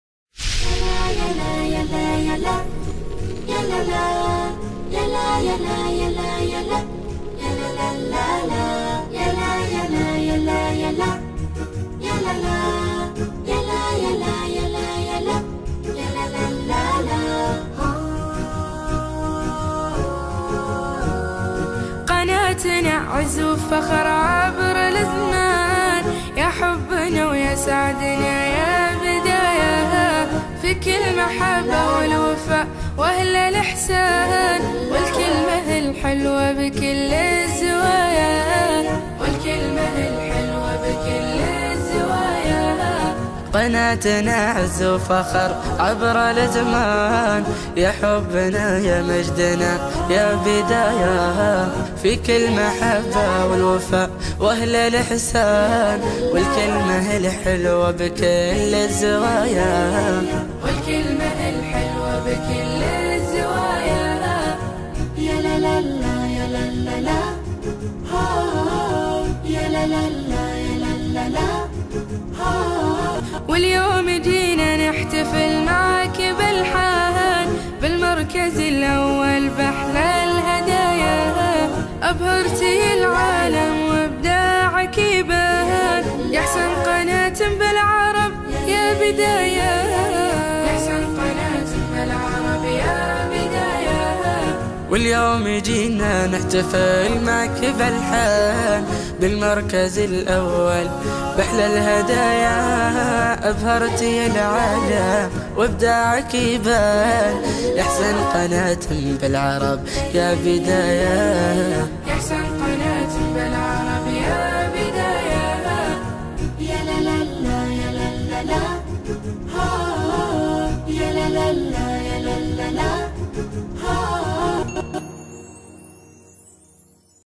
Public Loved it.
Educational Clip: Public Loved it. They thanked us for the small present, we gave them behalf of Muslim Community.
To support future community projects, please do donate per month as much as you can. Thank You